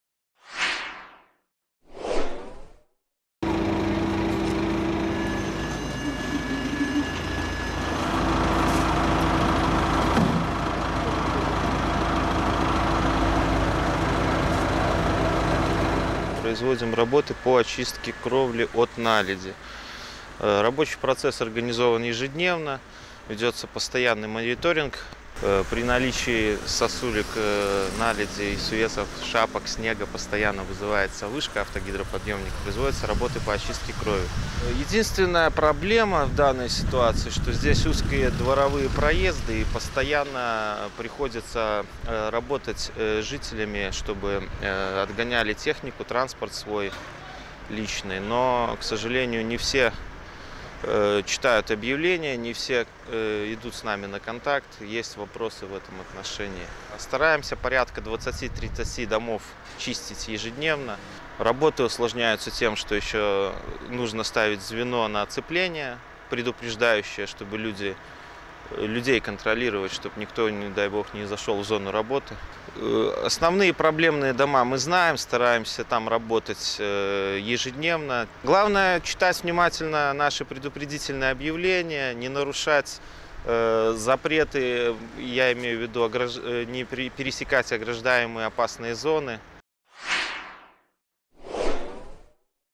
Как в Минске сбивают сосульки
Опасность зимой представляет любая наледь на крышах домов. С таянием снега размер сосулек будет только увеличиваться. Чтобы защитить горожан от нависшей над головами угрозы коммунальные организации Минска регулярно очищают крыши от наледи. Если вы заметили на доме сосульки, позвоните по единому номеру 115.